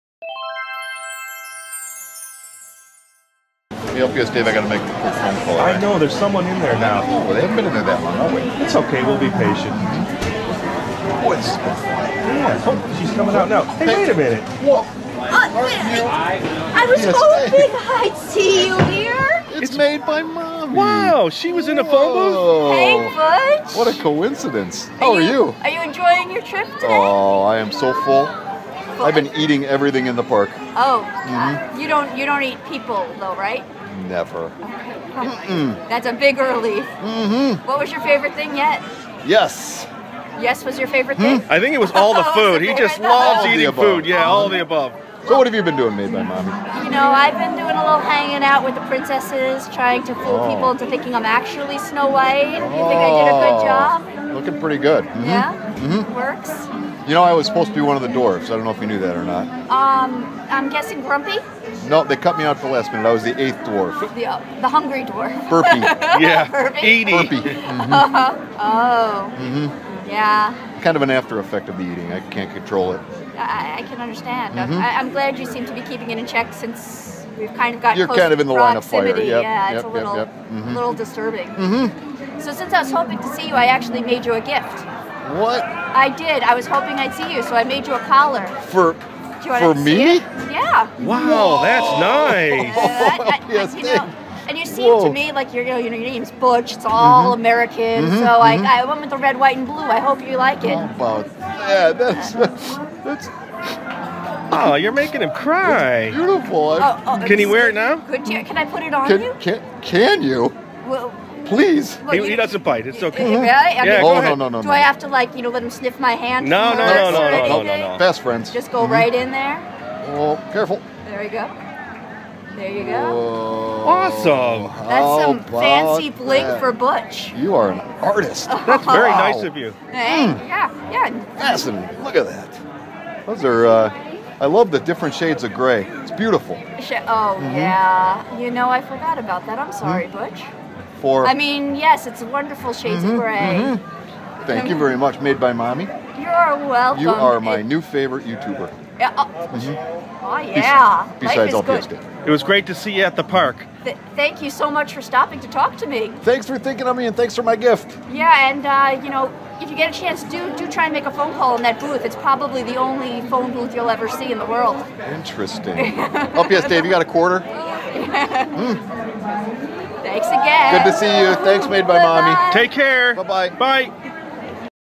You Found It! - Butch Gets a Gift From Made By Mommy at Disney's Epcot
​​​We had so much fun collaborating with Disney Parks and all the other YouTubers at the All Star Creator's Conference. Watch all the other Disney Side videos

What a "lucky" meeting!  Imagine Made By Mommy making a phone call!  We had been looking for her all day because she had a special necklace for Butch.  She sure had her Disney Side on!  Thanks again Made By Mommy!

Lucky Penny Thoughts: We were just about to get Fish-n-Chips from England when Butch wanted to check in with LPS-Jim by phone.  We had been looking for Made By Mommy all day and sure enough she was on the phone!  We had an email earlier in the week that she made a special necklace for Butch and was hoping to see him at Epcot.  It was cool that it all worked out!  Did you get a chance to find all of the "Hidden Mickey's?" Make sure you go back to the original video to find all three.

Later!
LPS-Dave

▶▶ Mobile Users!  Here are the bonus video links